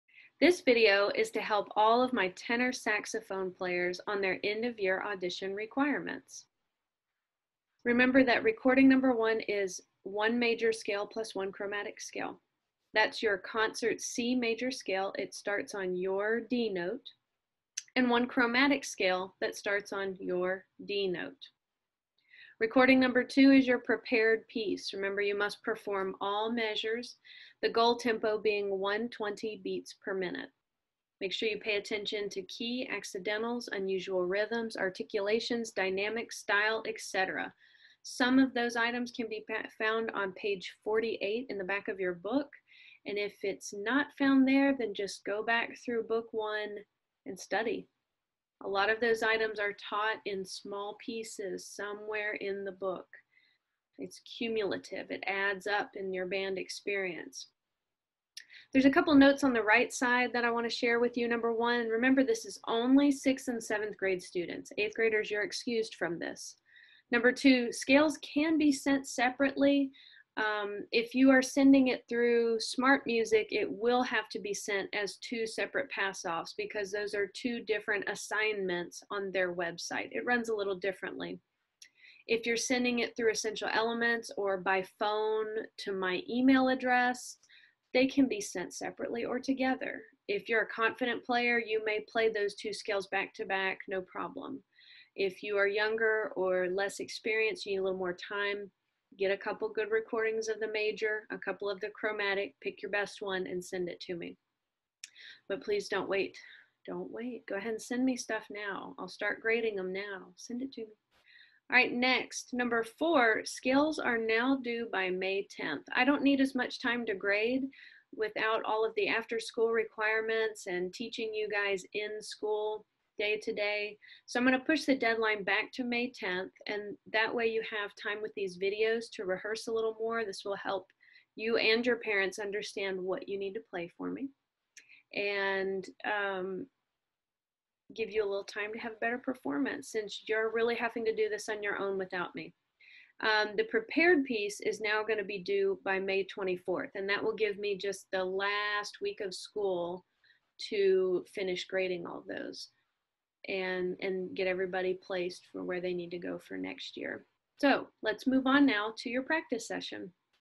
Tenor Audition Overview Only
Here is an overview of the end of year audition.  All resources used (and more!) are listed here in the description box.  Click SHOW MORE to see all.  :)
All scales should be performed in eighth notes, tongue up/slur down.

Major Scale Resources:
SmartMusic- Many of you will have to change the key signature.  To do so, click Display at the top, drop down menu to change the key.
Ms. Couey’s Scale Sheets-
Beginner 1 Octave Scale Sheet

Chromatic Scale Resources:
EEi- Fingering Chart- click Recording Studio, then Fingering Chart on the left side of screen.

Audition Piece:
Ms. Couey’s Printable PDF Files-
SmartMusic- this is a playable version online!!